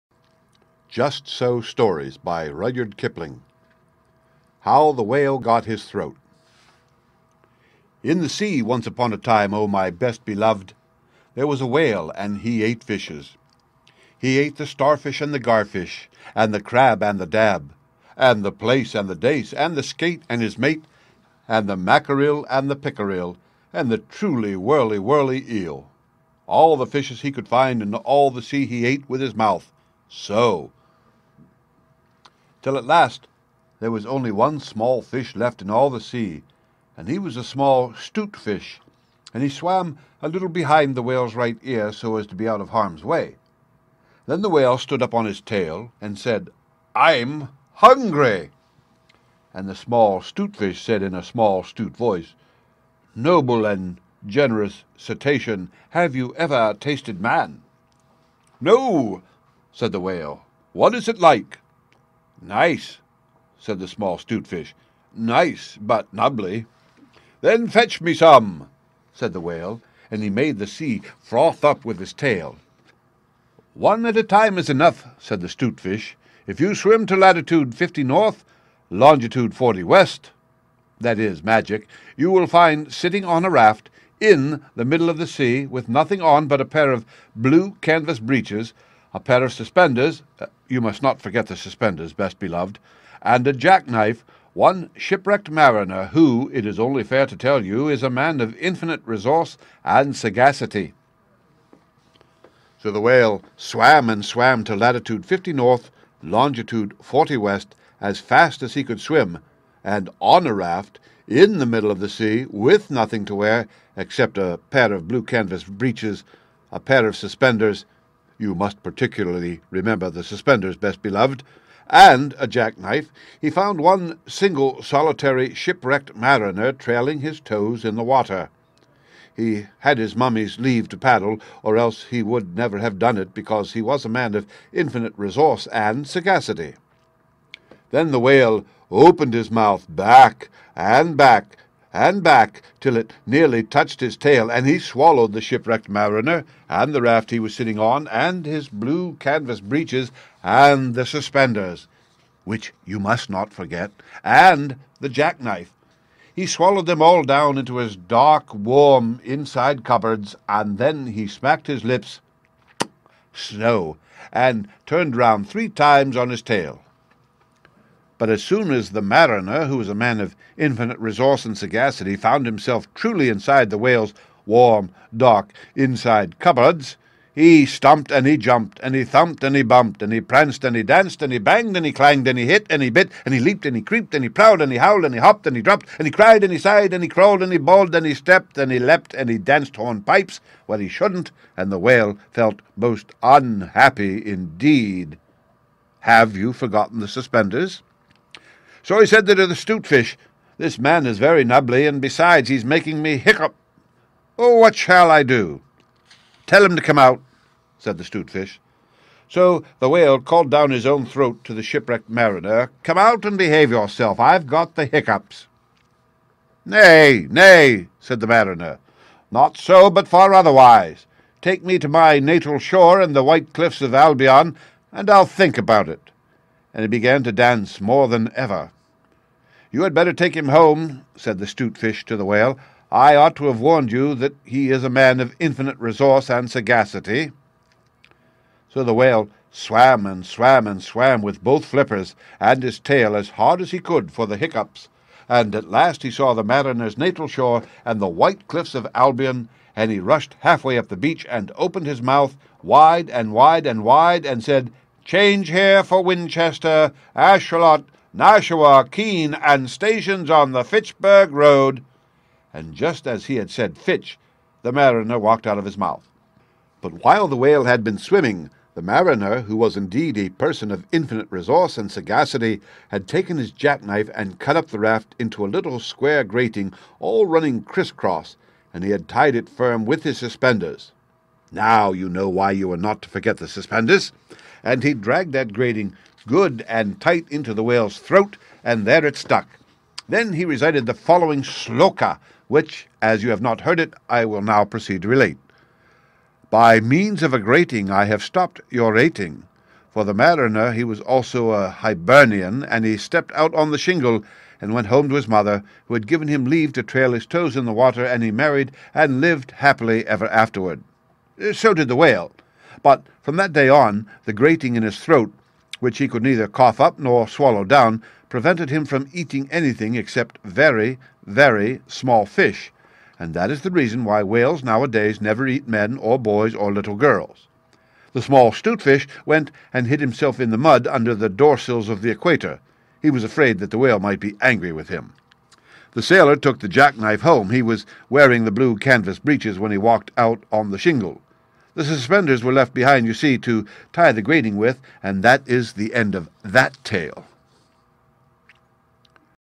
"HOW THE WHALE GOT HIS THROAT" Just So Storiesby Rudyard Kipling
story from Rudyard Kipling. The whale was once a fearsome predator who ate humans, but was changed by one of his victims.